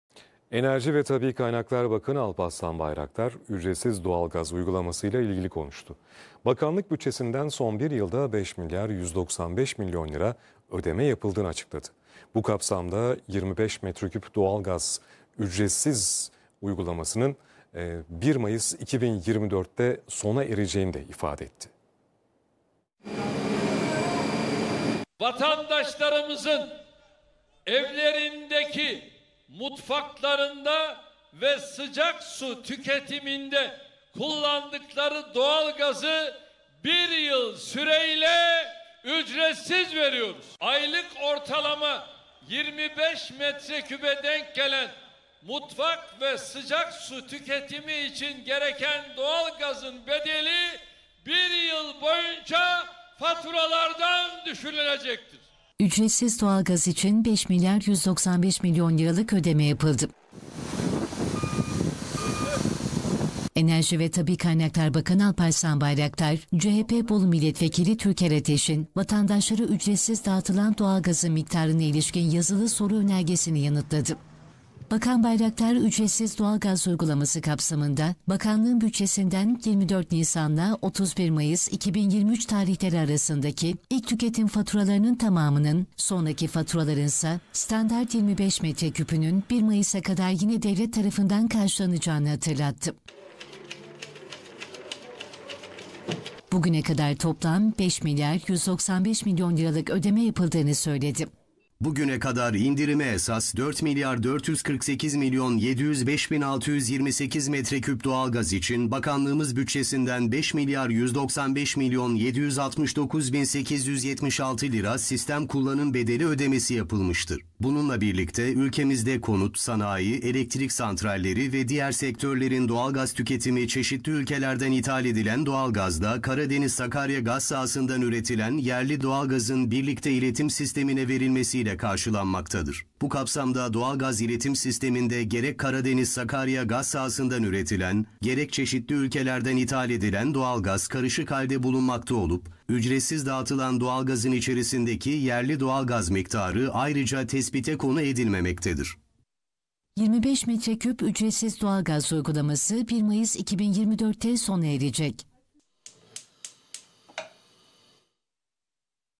Bayraktar'dan Ücretsiz Doğalgaz Açıklaması! Ücretsiz Doğalgaz Desteği Devam Edecek Mi? İşte Detaylar
Enerji ve Tabii Kaynaklar Bakanı Alparslan Bayraktar, ücretsiz doğalgaz uygulaması kapsamında Bakanlığın bütçesinden 24 Nisan 2023'ten bu ay başına kadar 5 milyar 195 milyon liralık ödeme yapıldığını söyledi.